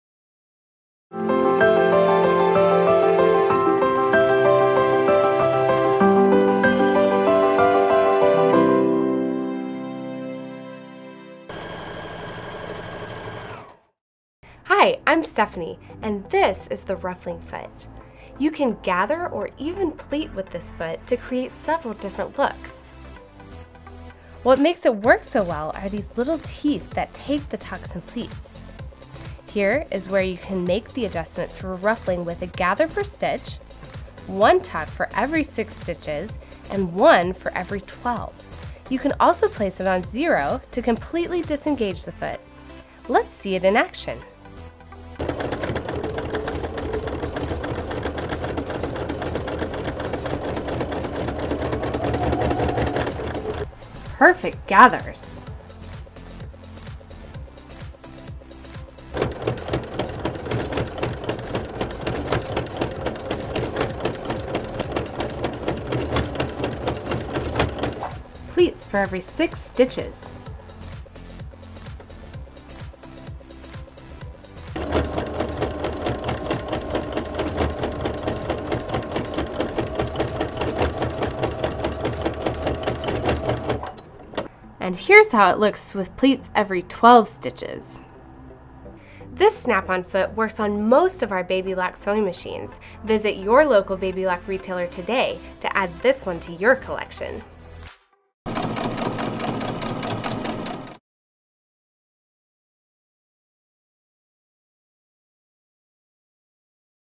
Baby Lock Accessory Spotlight: Sewing Machine Ruffling Foot
Easily make and attach ruffles as well as finish your edge in one simple step. Add perfectly spaced ruffles to garments, pillows, and other home decor items. This ruffler foot works on all fabric types.

*This foot is compatible with most Baby Lock sewing machines. Find a list of compatible accessories for your machines

Head over to Babylock.com for more ideas!